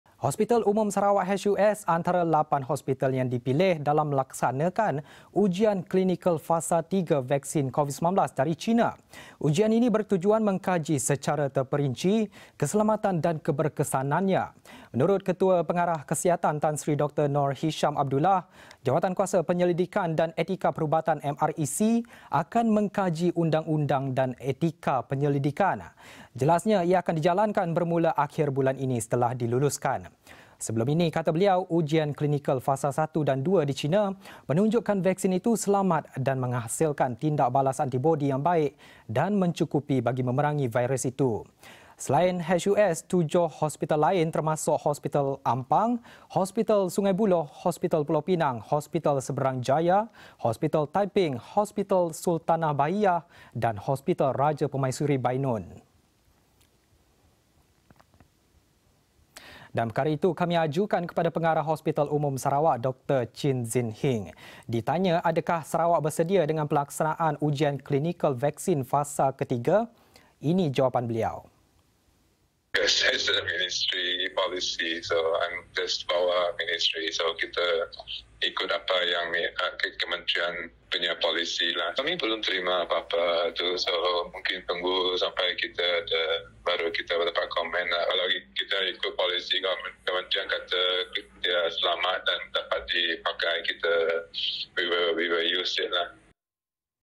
HUS dipilih sebagai lokasi ujian
Hospital Umum Sarawak (HUS) antara lapan hospital yang dipilih dalam laksana ujian klinikal fasa 3 vaksin COVID-19 dari China.
Ujian ini bertujuan mengkaji secara terperinci keselamatan dan keberkesanannya.